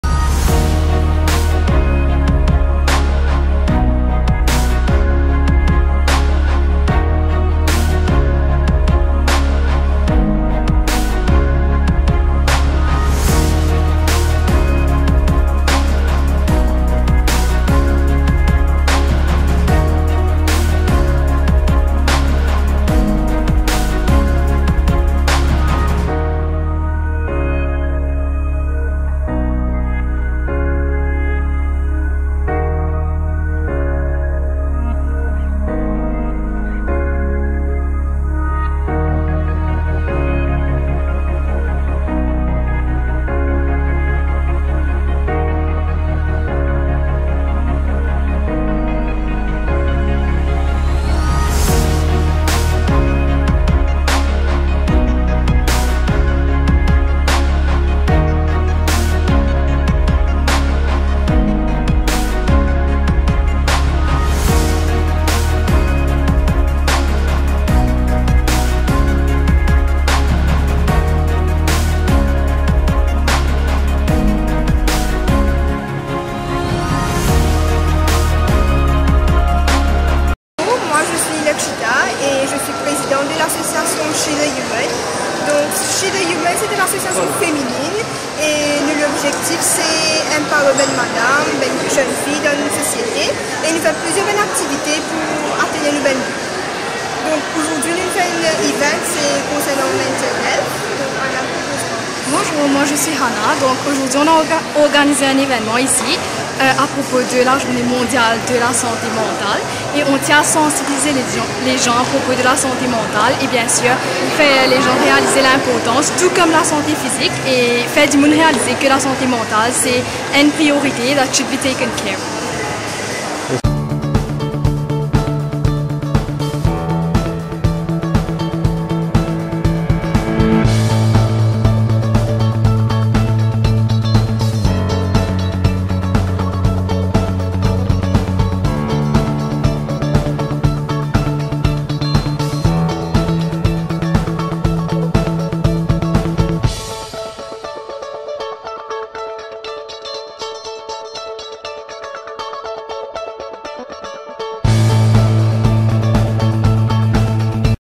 She The Human organise des activités autour de la santé mentale
L’association, She The Human a organisé des activités hier à Bagatelle Mall afin de sensibiliser les gens autour de la santé mentale. « Comme le fait d'être en bonne santé physique, être en bonne santé mentale permet de se sentir bien dans sa peau. Cela permet également de profiter des joies de la vie, de s'épanouir et d'essayer de nouvelles choses », affirme Hannah, la responsable de communication de She The Human.
La Présidente, Lakshita Ramroop nous a fait ressortir que l’objectif de leur association est de responsabiliser et aider les filles et femmes de s’épanouir.
A noter que la World Mental Health Day 2021 est célébrée aujourd’hui, ce dimanche, 10 octobre à travers le monde.
La dépression est une maladie courante dans le monde. On estime que 3,8 % de la population est touchée, dont 5,0 % d'adultes et 5,7 % de personnes âgées de plus de 60 ans (1). À l'échelle mondiale, environ 280 millions de personnes souffrent de dépression.
Le Plan d’action pour la santé mentale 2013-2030 de l’OMS met en évidence les étapes nécessaires pour fournir des interventions appropriées aux personnes atteintes de troubles mentaux, dont la dépression.